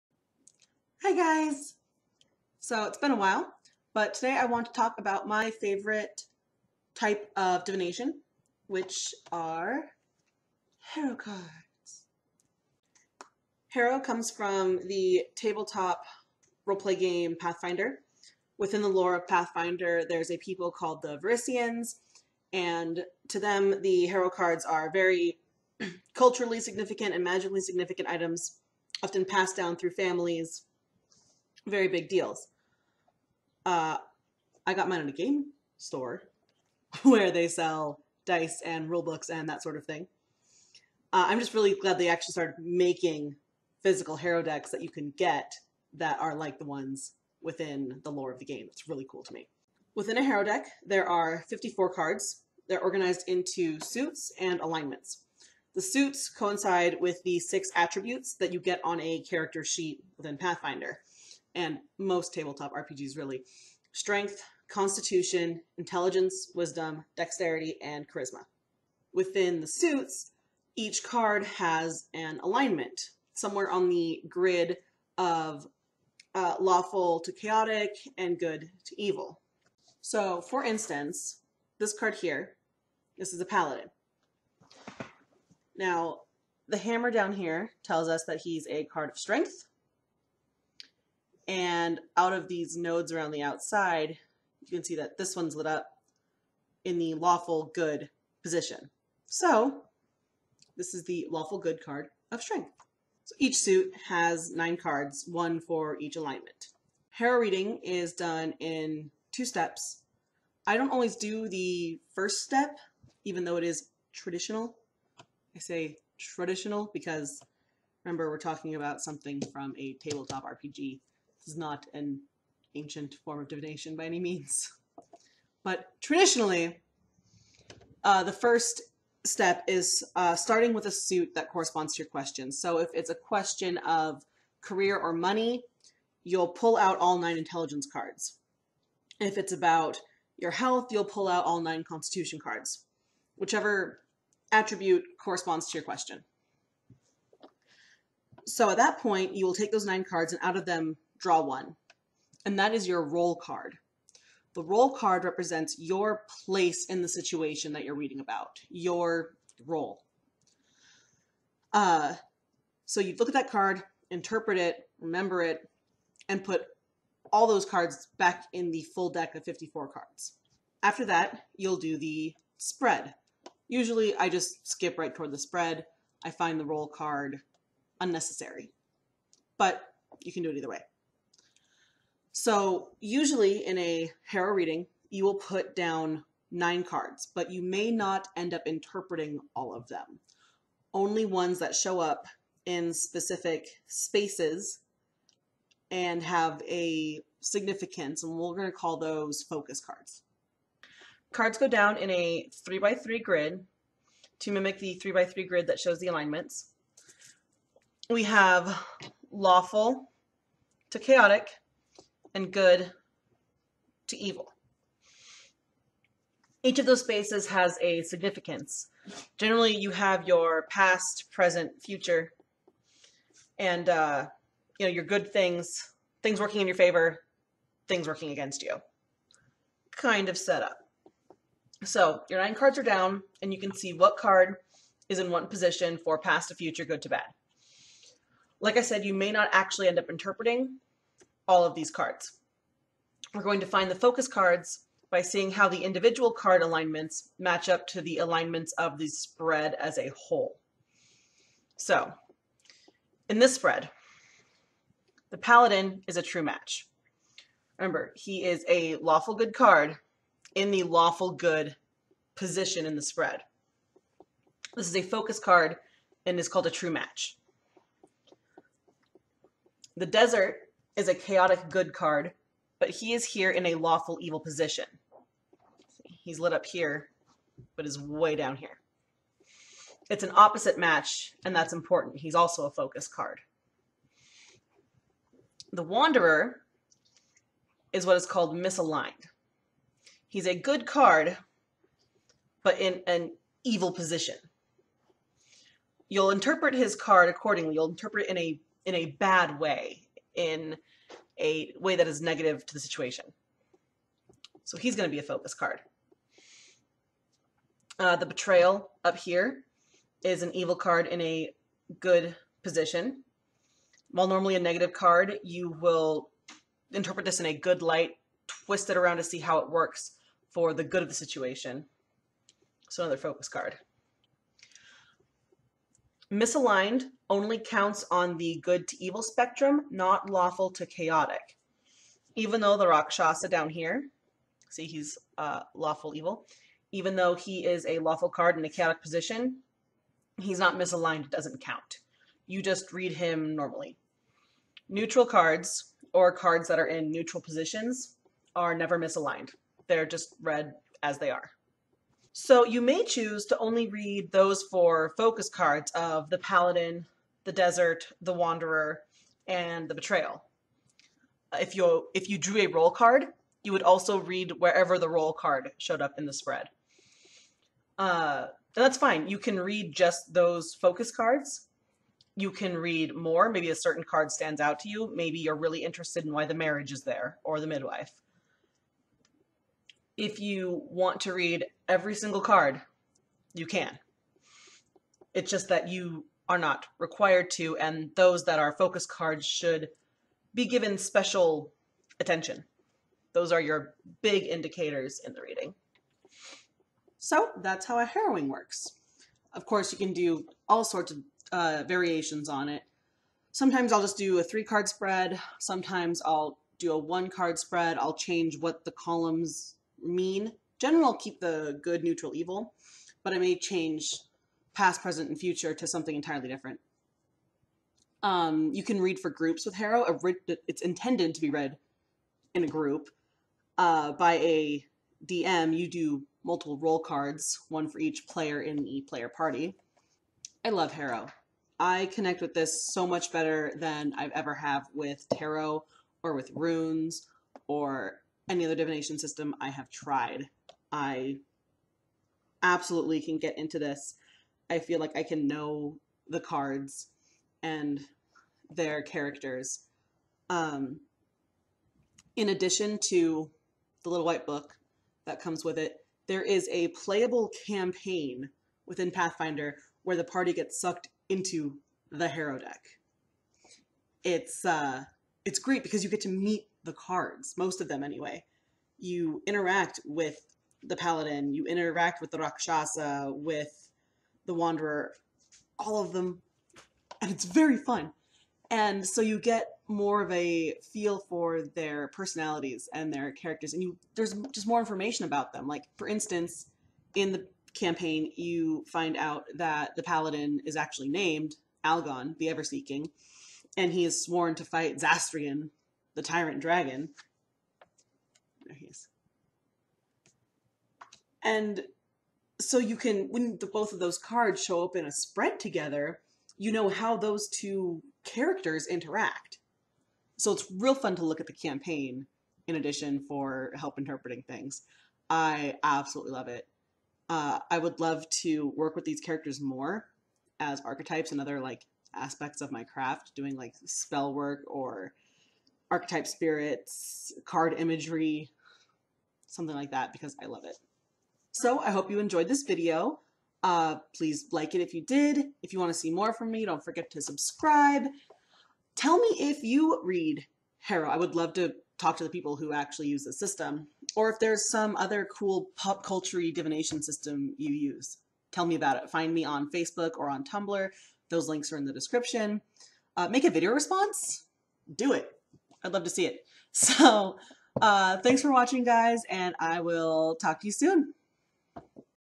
Harrow Card Divination
Harrow Cards from the tabletop RPG Pathfinder! My favorite system of divination!